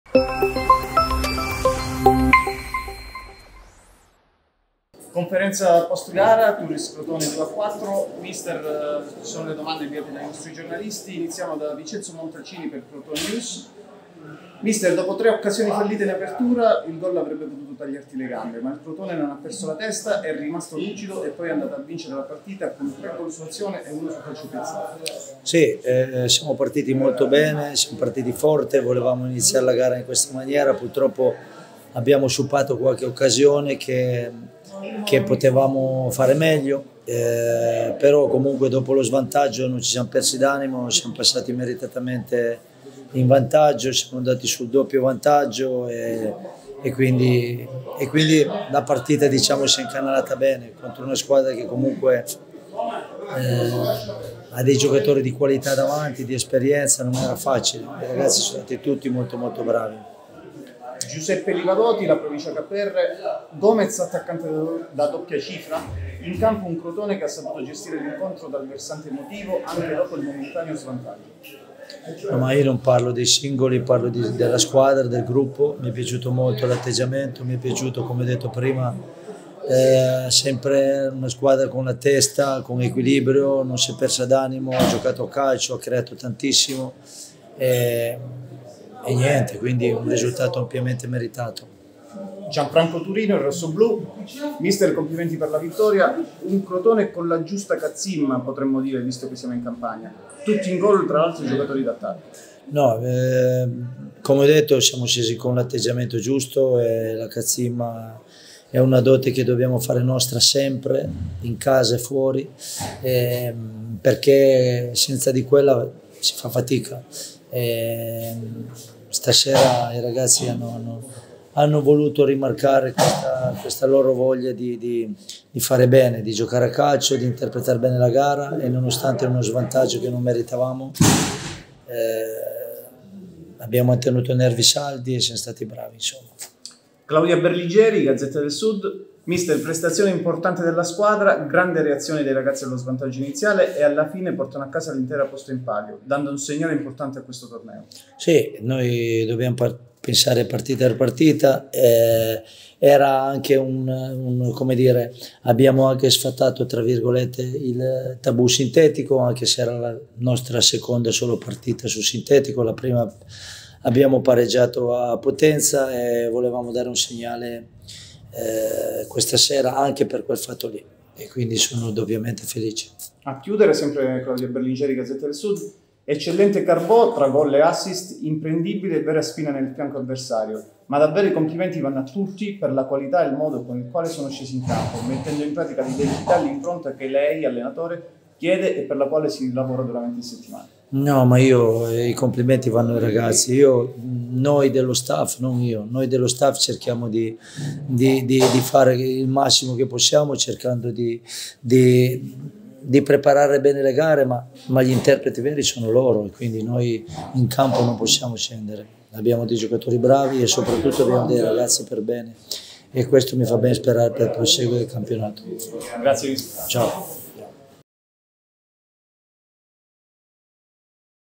Turris-Crotone, mister Lerda nel post gara
Le dichiarazioni di mister Lerda al termine di Turris-Crotone